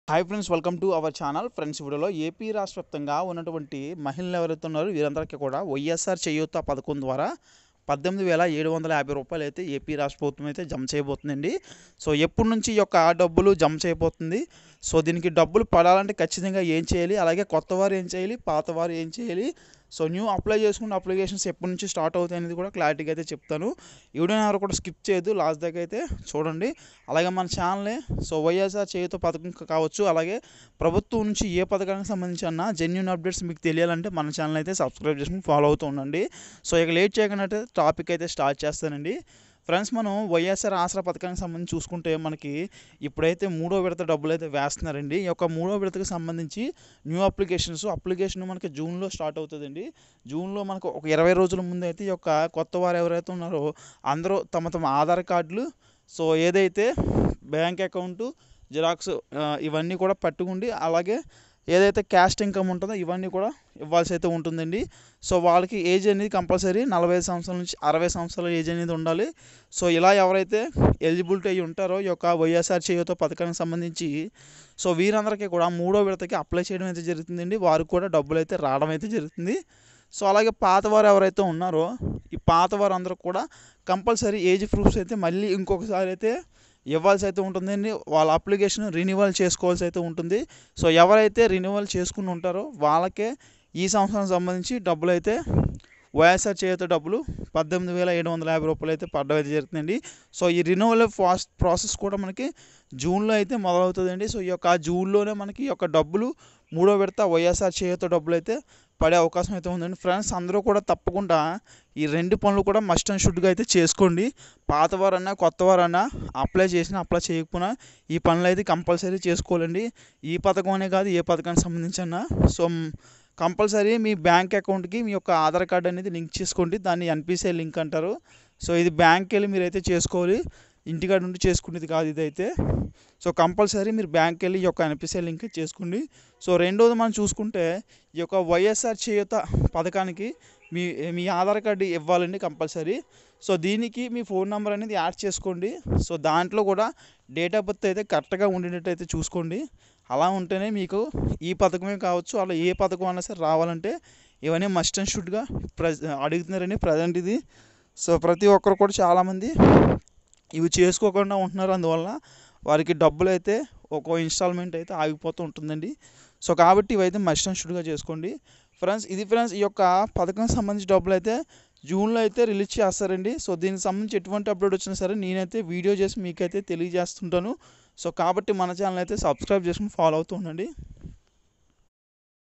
వైస్సార్ చేయూత మూడో విడతకి డేట్ ఫిక్స్ వెంటనే ఈ పత్రాలు దగ్గర పెట్టుకోండి 2023
Ysr cheyutha date fix
Ysr cheyutha 3rd installment updates
Ysr cheyutha updates